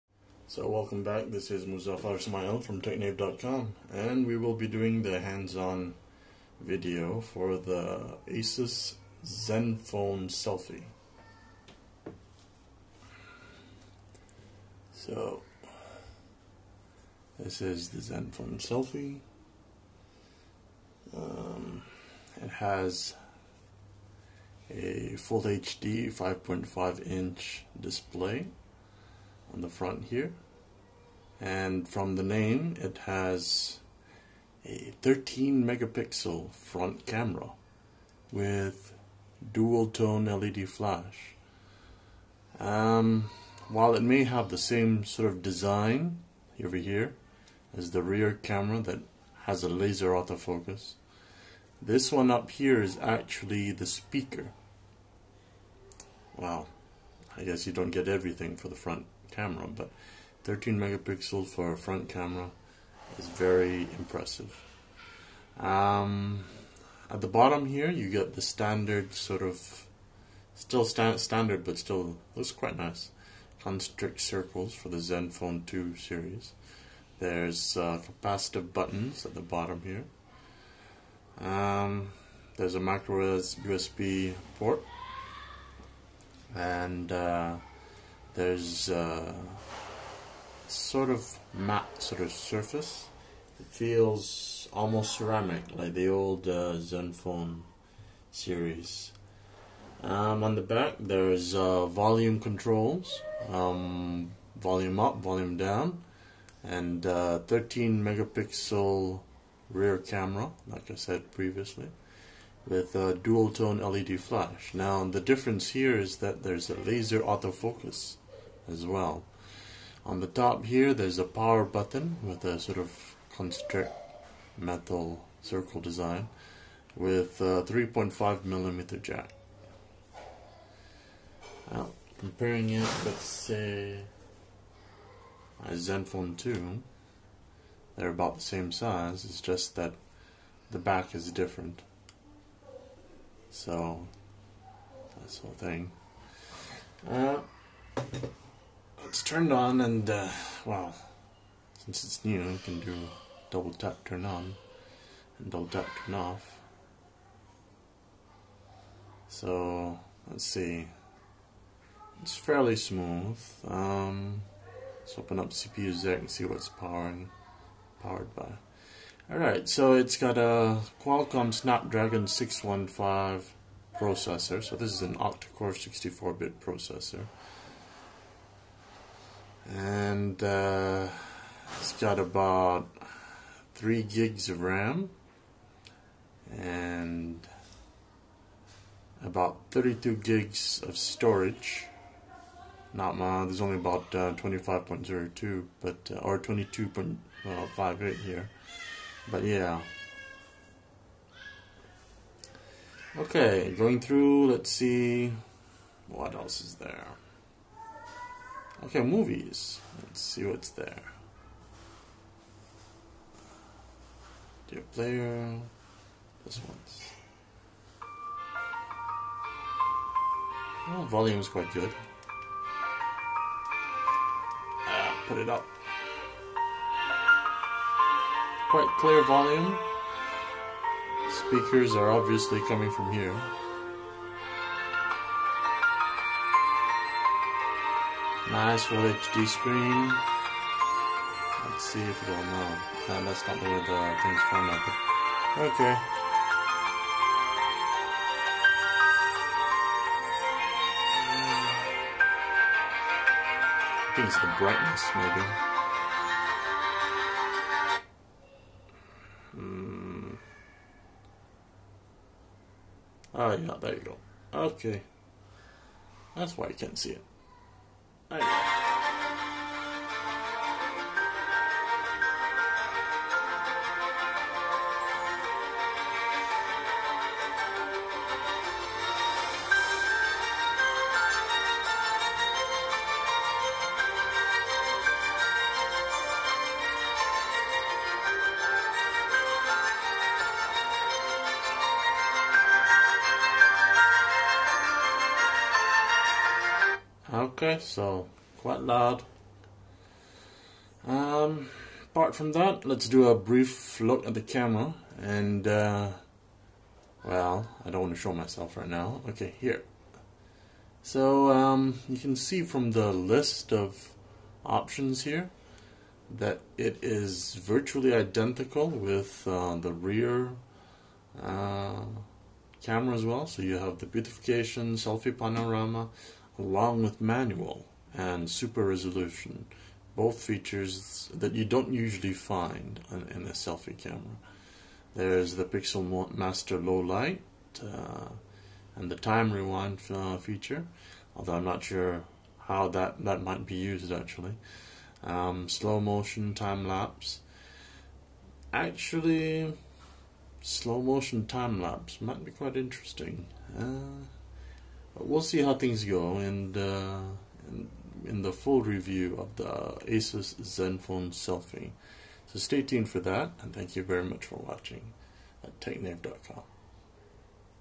ASUS ZenFone Selfie hands-on at TechNave.com
This is the hands-on video for the ASUS ZenFone Selfie. Priced at RM1049 this smartphone packs in 13MP front and back cameras both with dual tone LED flash.